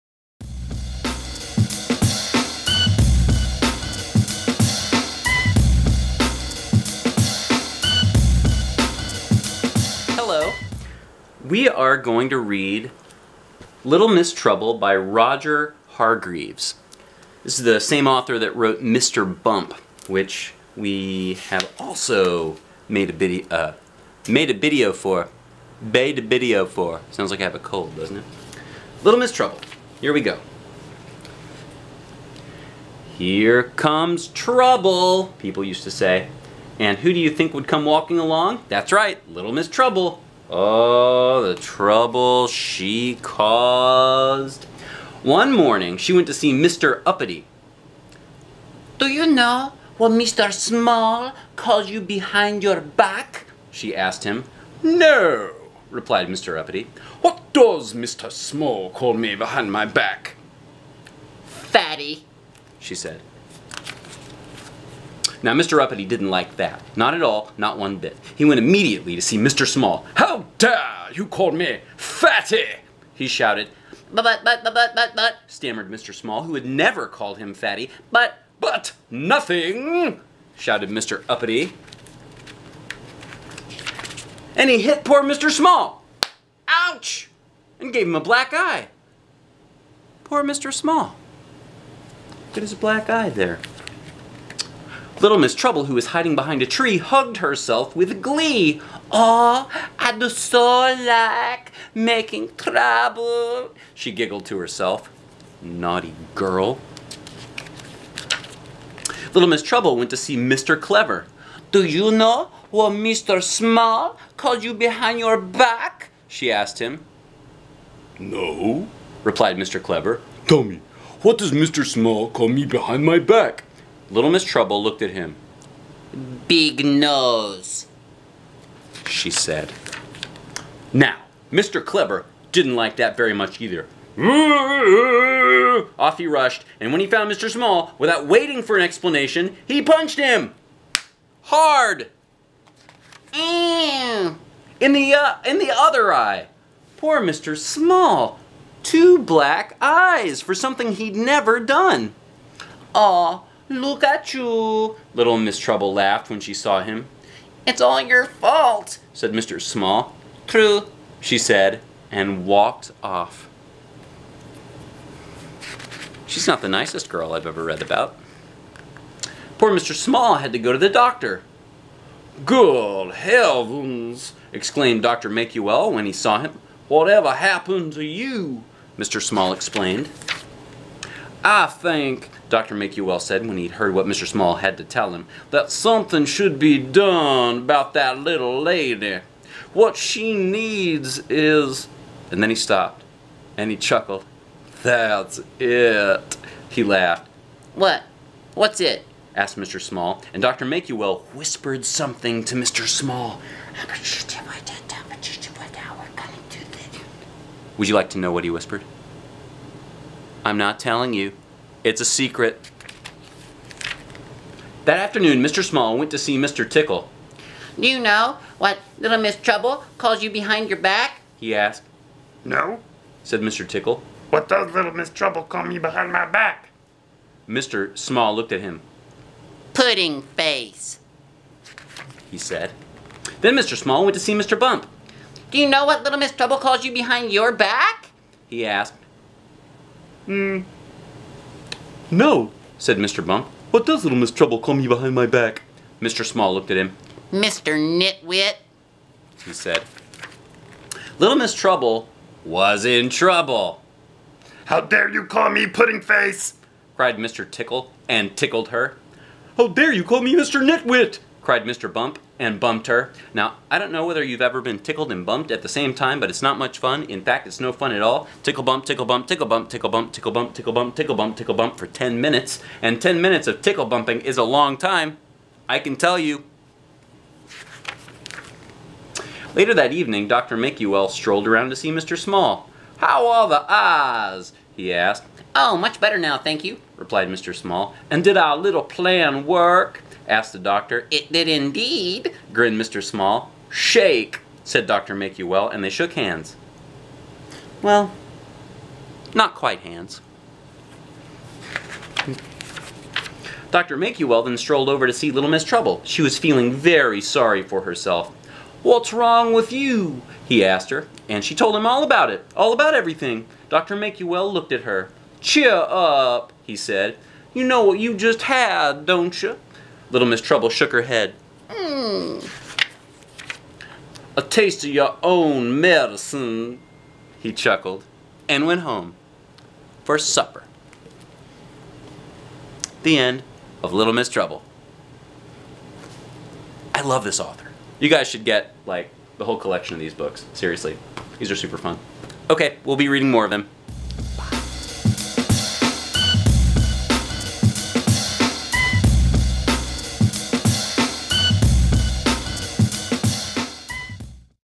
Little Miss Trouble - a book read aloud by a dad - from Seriously, Read a Book!
Uh-oh! It's time for Little Miss Trouble to get a taste of her own medicine.